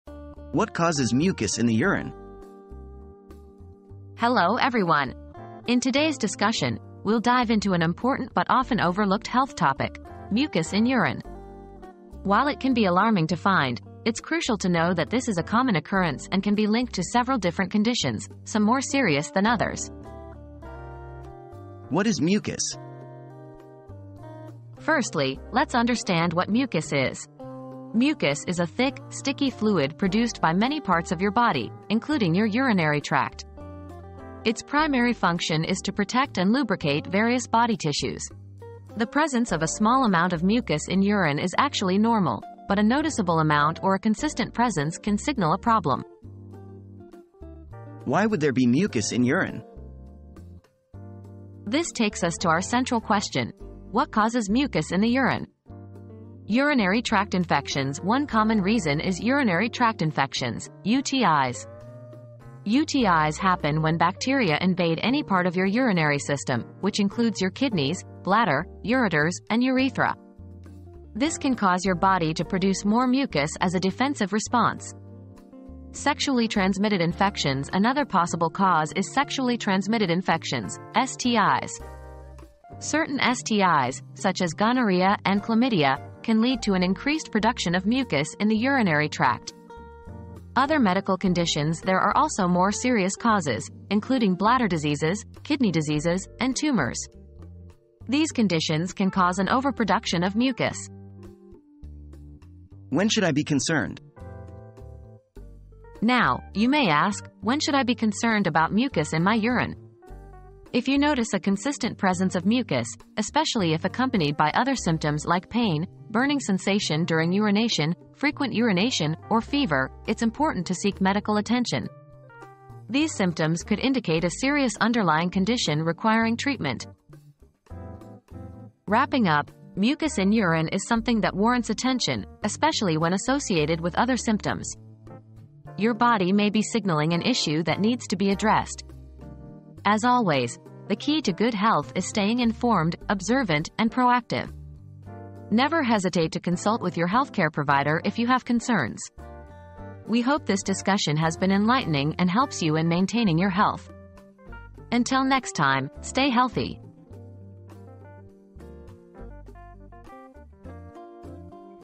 What causes mucus in the urine?
Unmasking the Mystery: Mucus in Urine - Causes and Concerns • Unmask the mystery of mucus in your urine! Learn about the causes and concerns of this common occurrence in our latest video. From urinary tract infections to more serious medical conditions, find out when you should be concerned and seek medical attention. Don't miss out on this important health topic!

00:00 • What causes mucus in the urine?
00:21 • What is Mucus?
00:48 • Why Would There Be Mucus in Urine?
01:42 • When Should I Be Concerned?